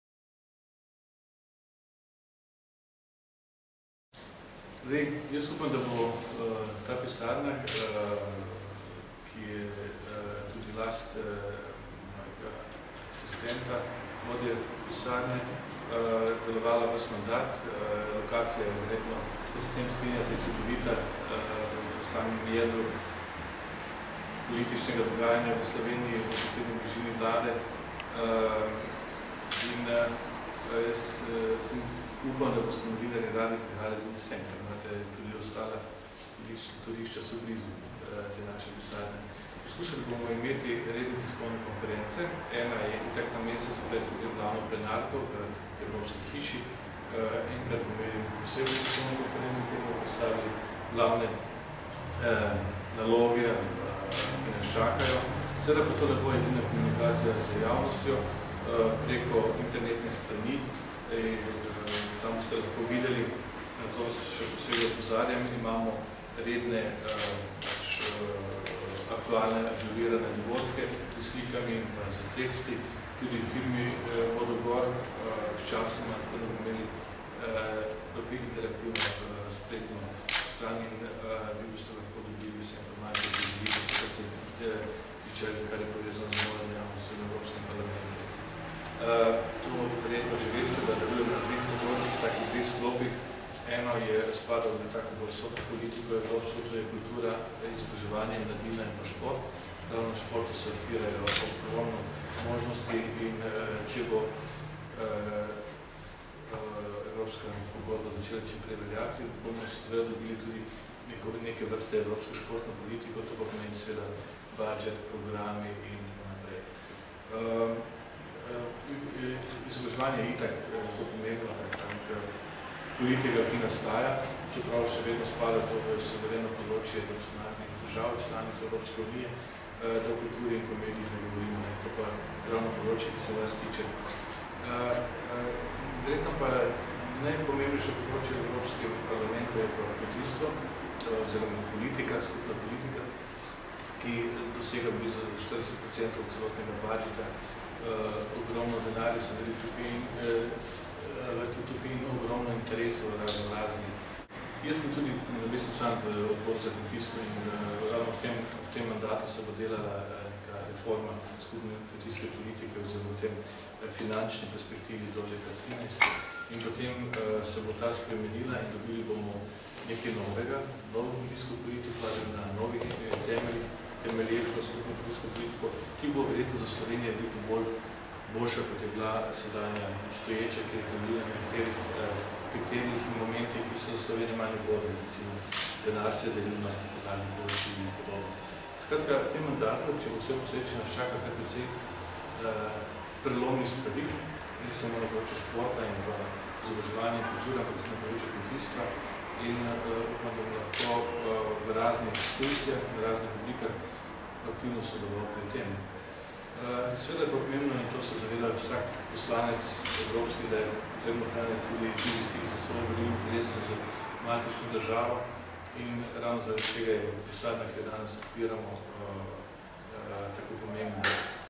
Otvoritev pisarne v Ljubljani
Evropski poslanec dr. Milan Zver je v petek 9. oktobra v Ljubljani odprl pisarno.